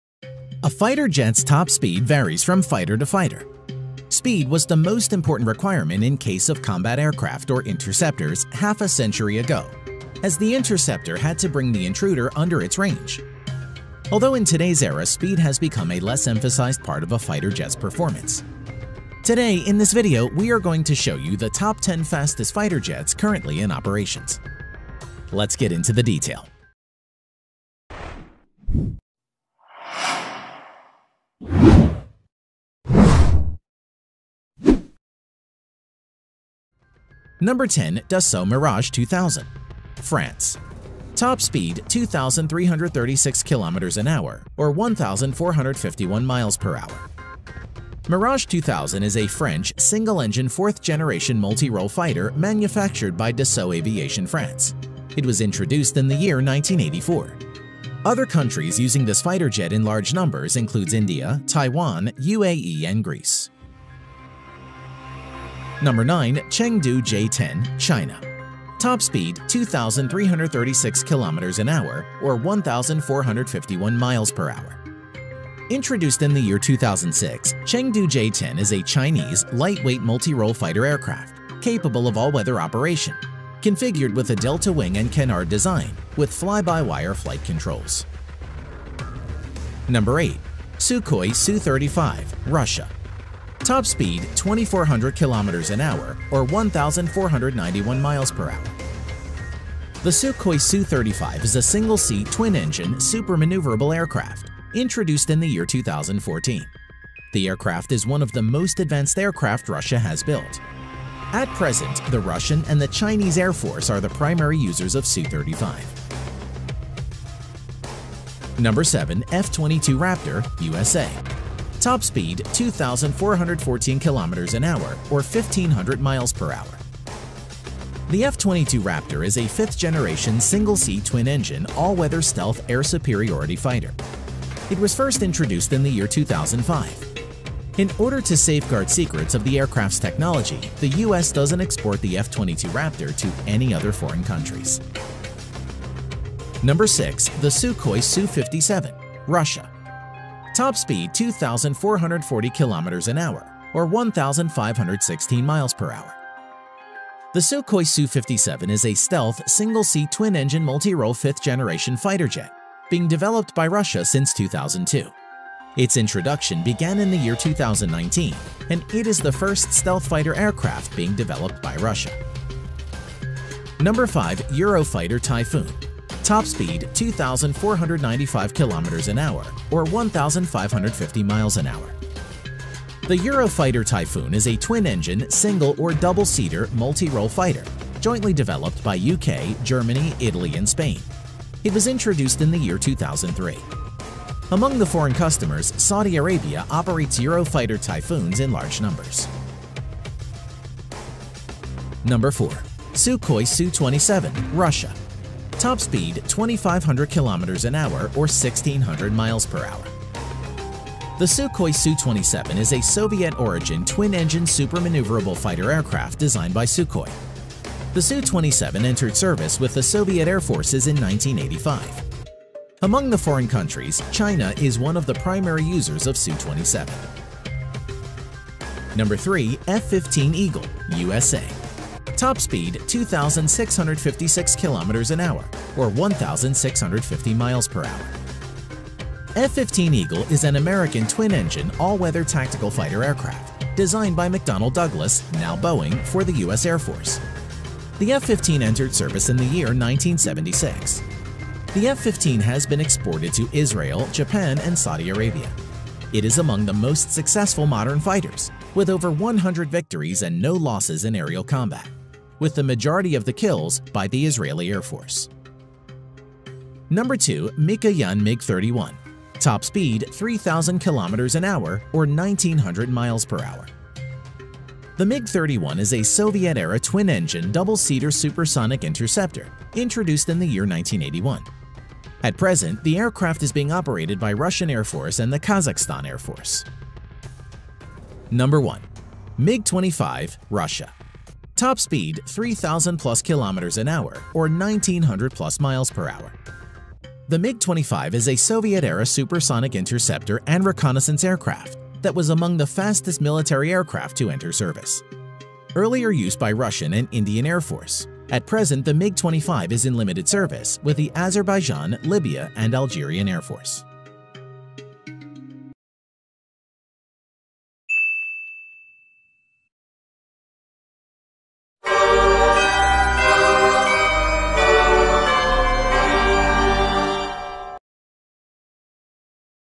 Top 10 Fastest Fighter Jets (Video footage +Narration) | CREATIVE COMMON CHANNEL
Today in this video we are going to show you the top 10 fastest fighter jets currently in operations. A fighter jet's top speed varies from fighter to fighter.   Speed was the most important requirement in case of combat aircraft (interceptors) half a century ago as the interceptor had to bring the intruder under its range.Although  In today's era, speed has become a less
emphasized part of a fighter jet’s performance.
Number​ 10:Chengdu J-10, China
Number 9: Dassault Mirage 2000 France
Number​ 8:Sukhoi Su-35, Russia
Number​ 7 : F22 Raptor, USA
Number 6: The Sukhoi Su-57
Number 5:Eurofighter Typhoon
Number​ 4 : Sukhoi Su-27, Russia
Number​ 3: F-15 Eagle,USA
# Number 2: Mikoyan MiG-31
Number​ 1: Mig 25 Russia

The mig 25 is a soviet era supersonic interceptor and reconnaissance aircraft that was among the fastest military aircraft to enter service.

Earlier use by Russian and Indian Air force, at present the Mig 25 is in limited service with the Azerbaijan,Libya and Algerian Air force